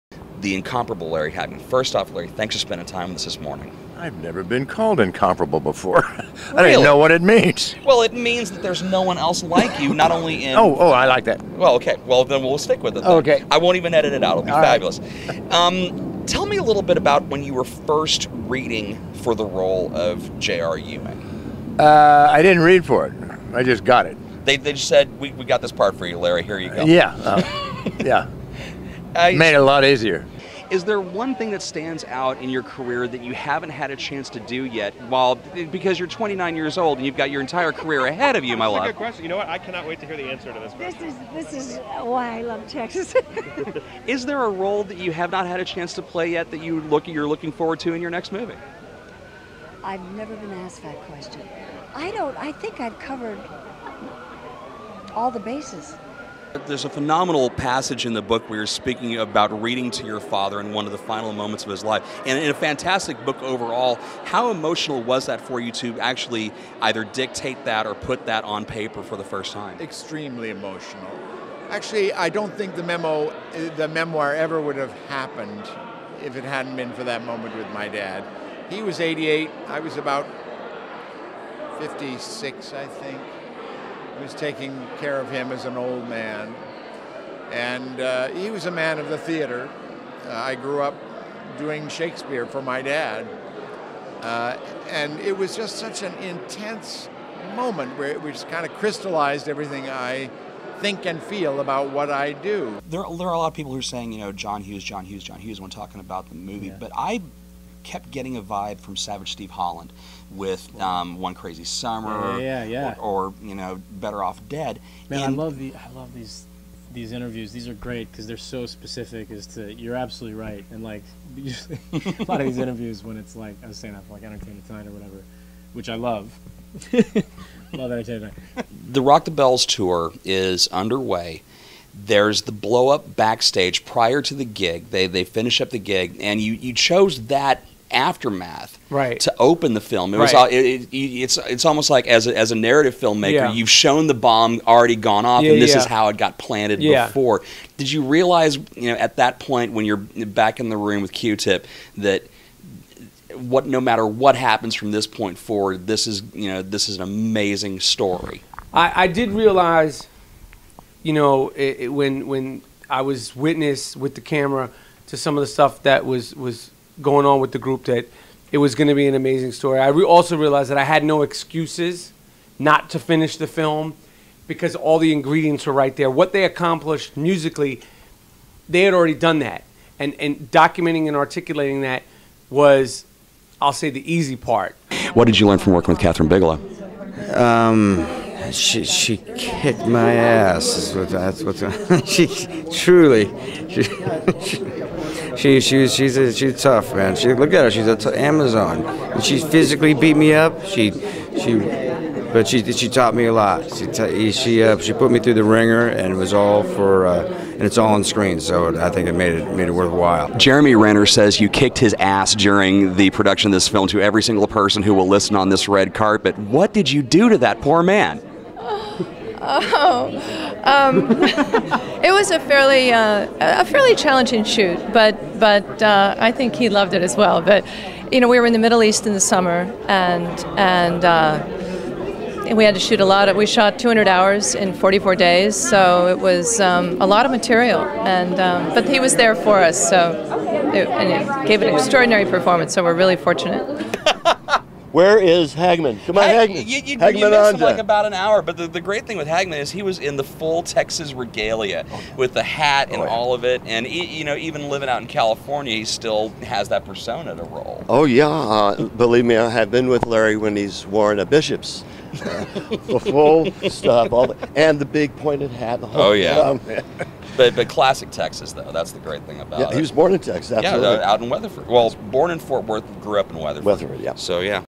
Devin Pike: Host / Emcee Reel
Conversations with Larry Hagman, Ann-Margret, John Lithgow, Topher Grace, Michael Rapaport, Jeremy Renner, Kathryn Bigelow